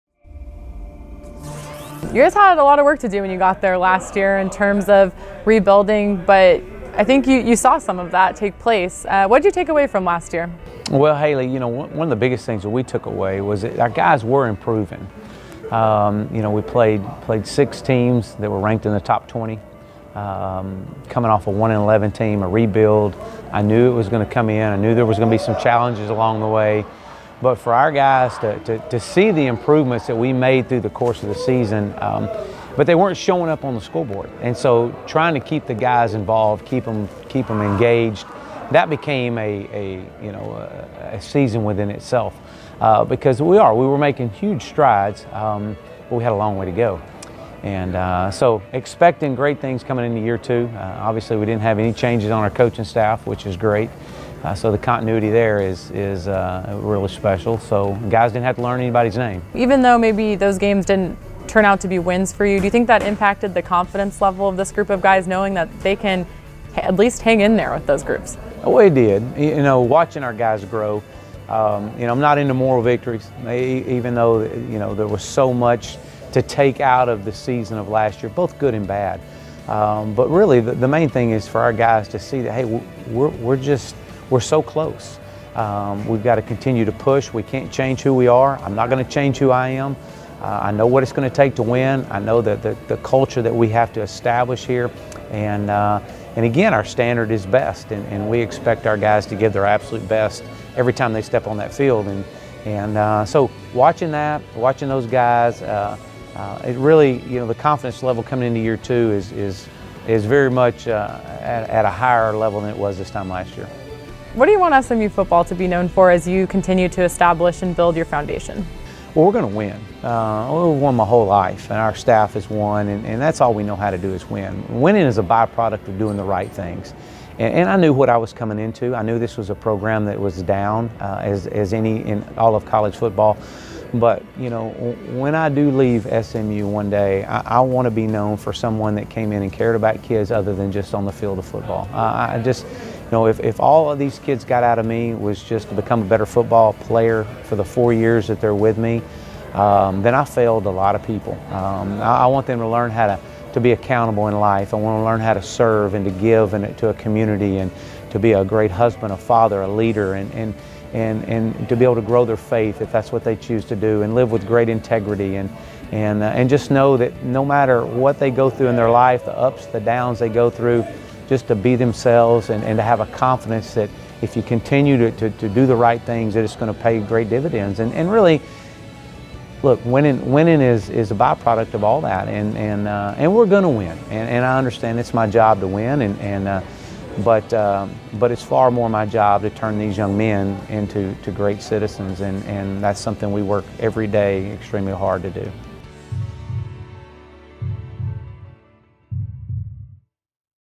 One-On-One with SMU Head Coach Chad Morris
The American Digital Network's Hali Oughton sat down with SMU Head Coach Chad Morris at Football Media Days to discuss the 2016 season.